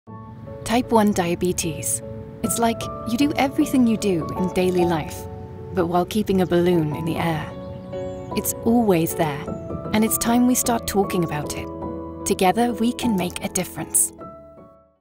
The Story of the Blue Balloon
Life is a constant balancing act, but life with diabetes can be even more of a challenge. It’s like doing everything you do day to day, while keeping a balloon in the air, and it’s time we start talking about it. Together, we can make a difference.